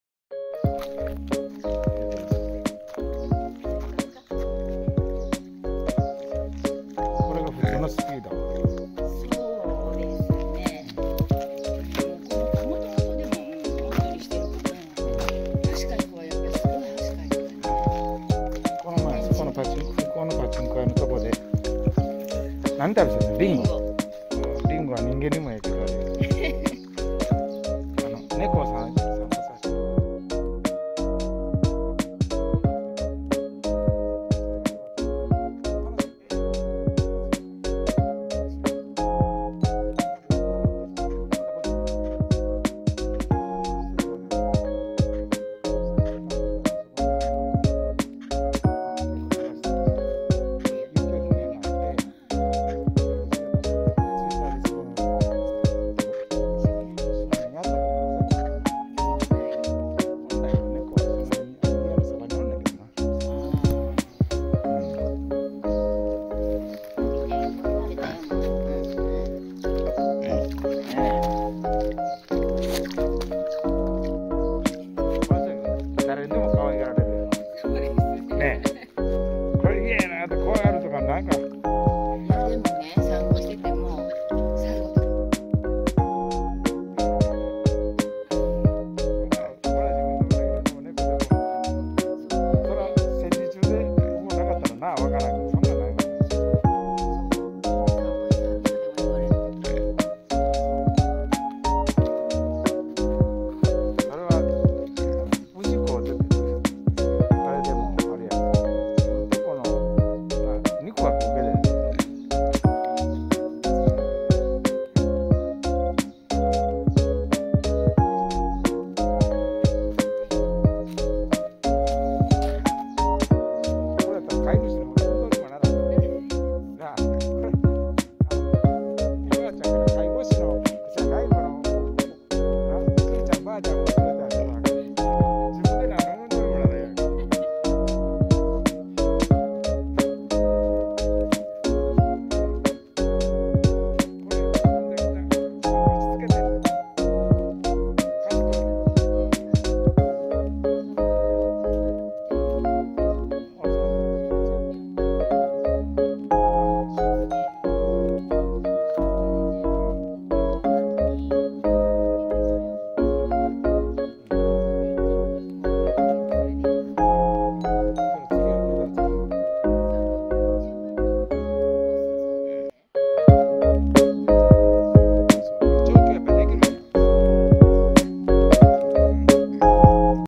ミニ豚の散歩 ビッキー16歳（メス）堺市北区:
堺市駅の東側を朝夕２回散歩に連れています。スピードがゆっくりです。
　出会ったら声をかけてくださいね